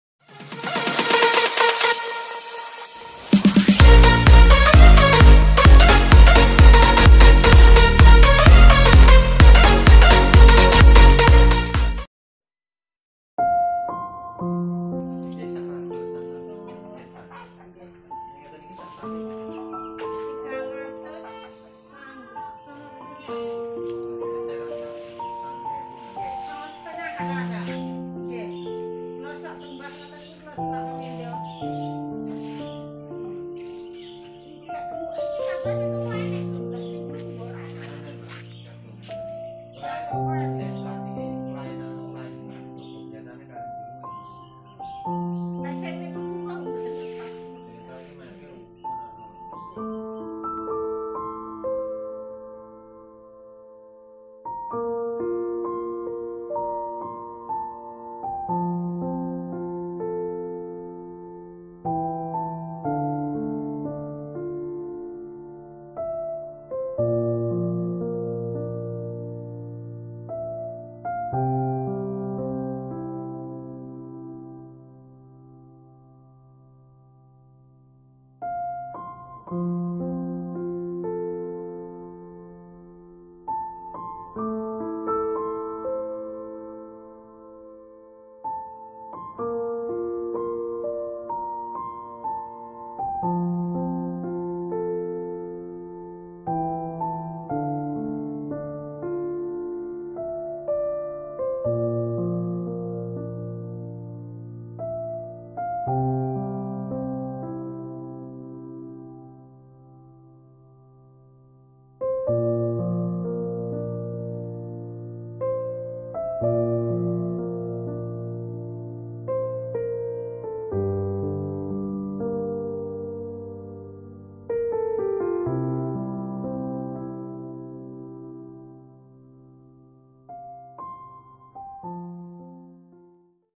Pertanian dan Peternakan Sapi Brahman
hampir mayoritas masyarakat petani atau buruh tani di kampung saya selain bertani juga beternak. peternakan sapi ataupun kambing. karena untuk pakanya dari jerami padi. untuk menambah penghasilan rata2 mereka juga beternak.